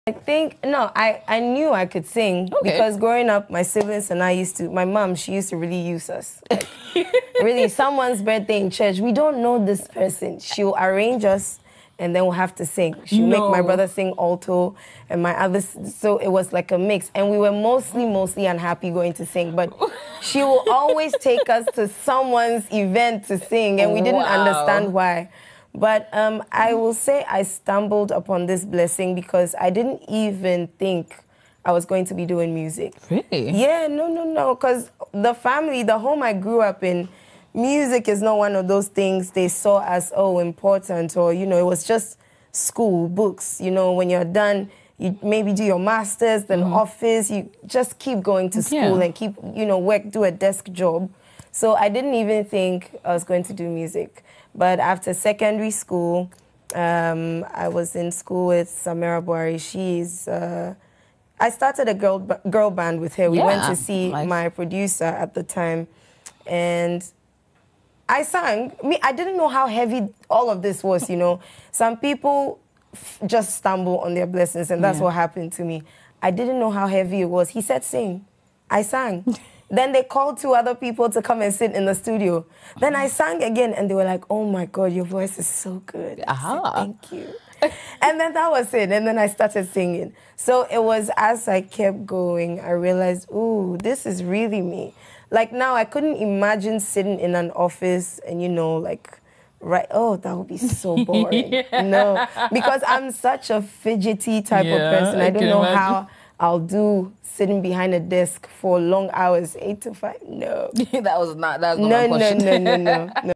I stumbled into my music career - MzVee | Hall of Fame
Ghanaian musician, MzVee, talks about how she discovered her voice and glided into music to pursue a career.